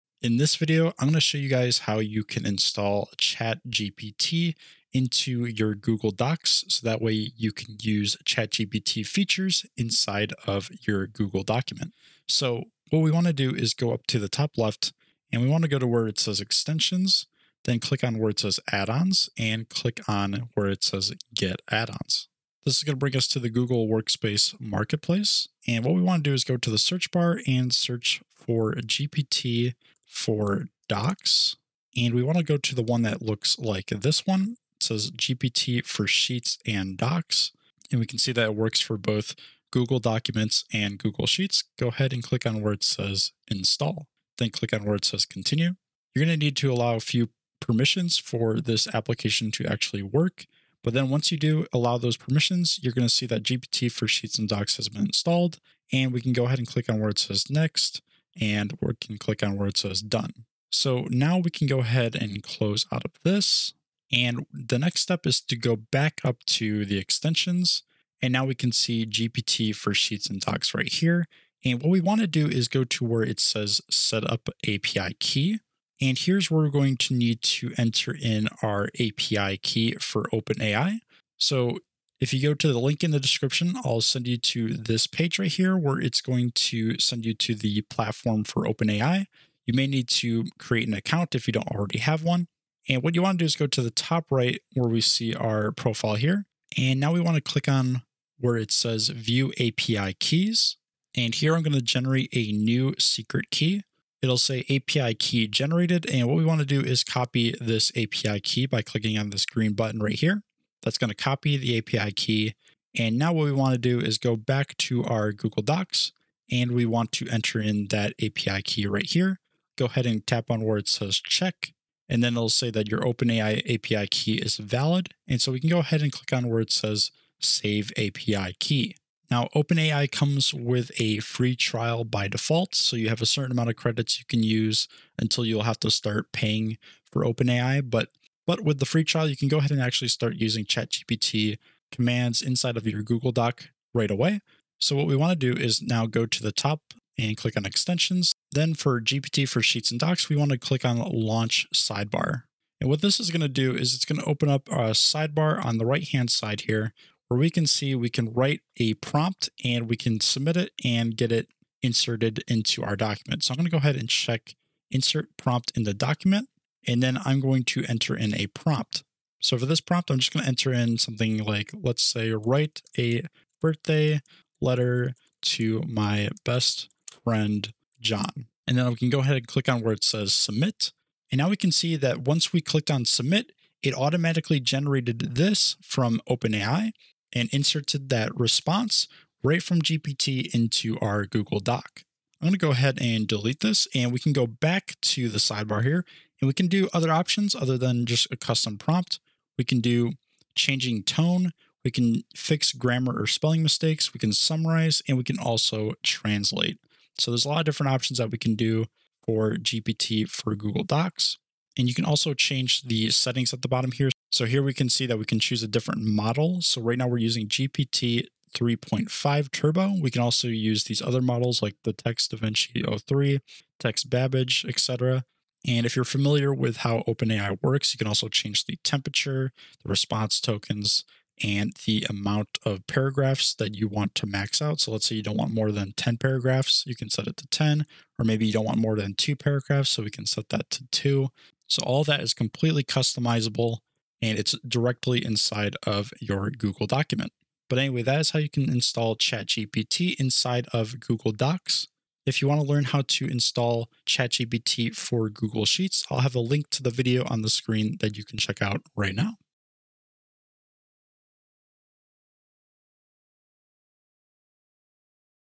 How to Install Chat GPT for Google Docs - Use GPT AI in Documents
In this video we go over how to use chat gpt with google docs. You can use OpenAI's GPT as functions inside of Google Documents to help improve your workflow.